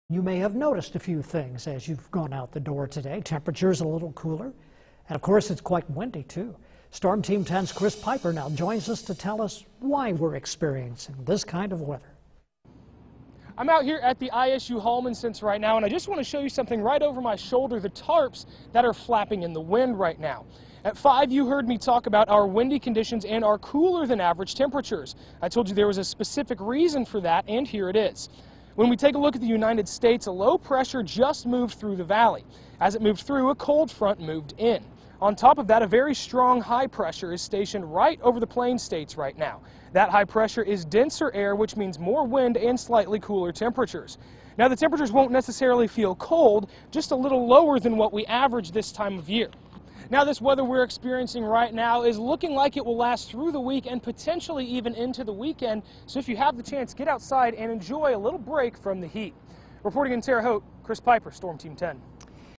Windy Start to the Week
There's a reason we're windy, and cooler than average right now.